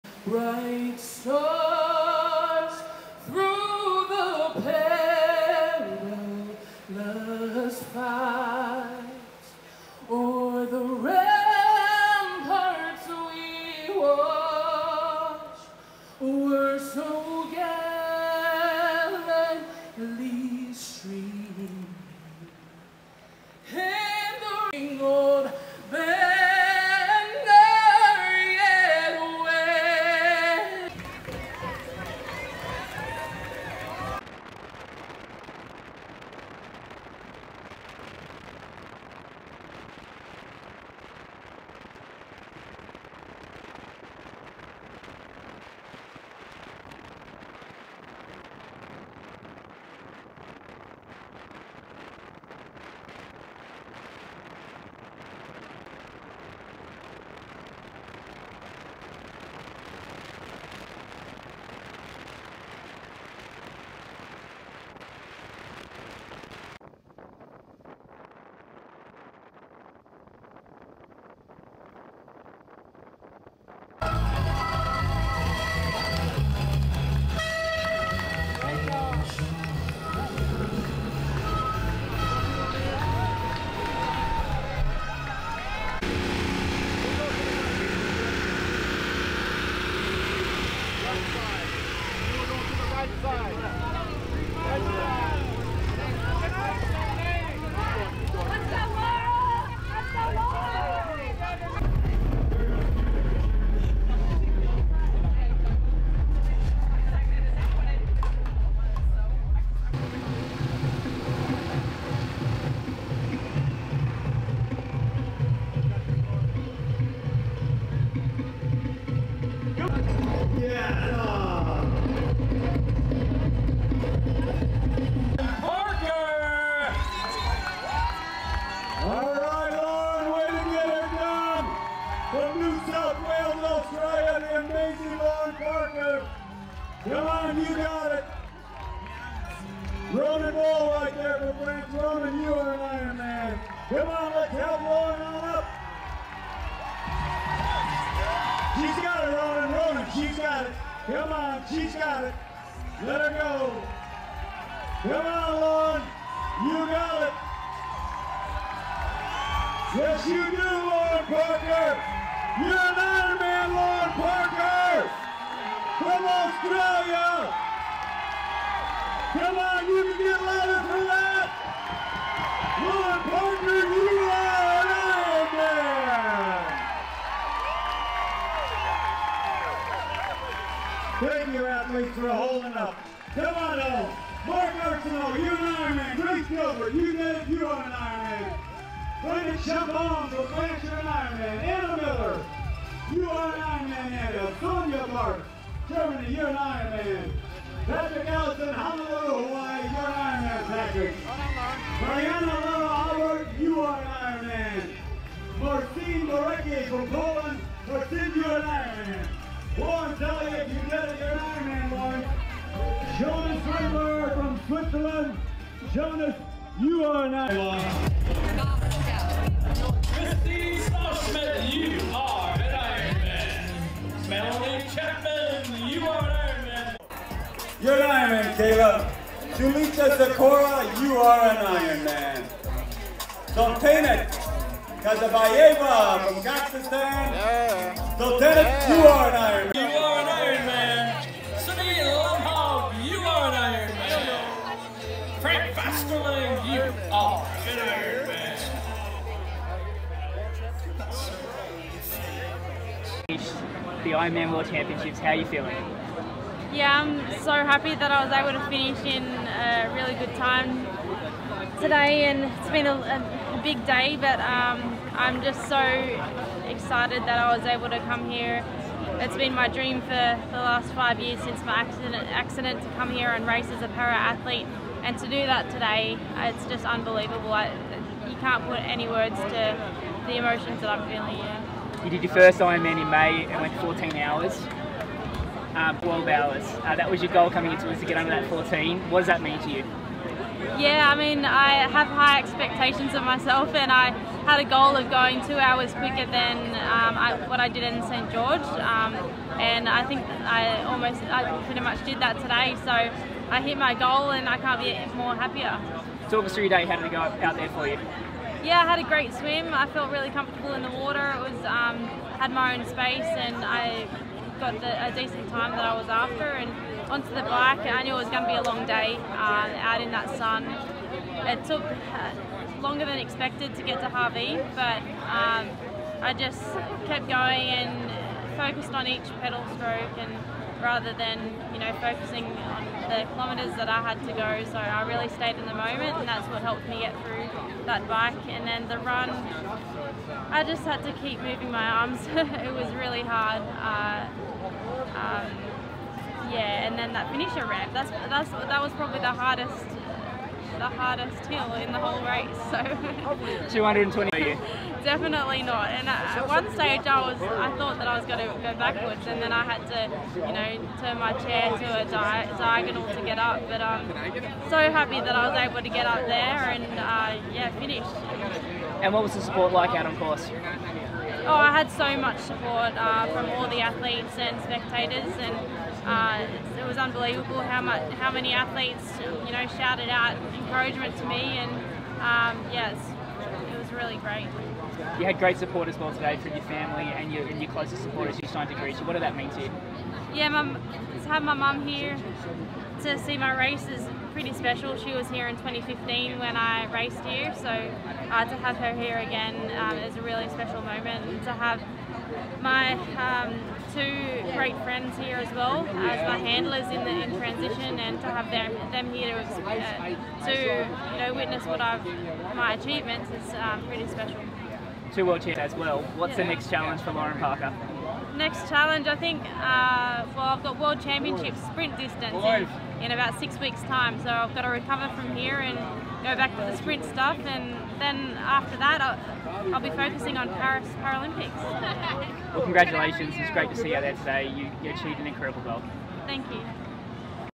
Lauren Parker - Ironman World Championship 2022
We follow Lauren Parker through her swim, bike and run during the 2022 Ironman World Championship race in Hawaii.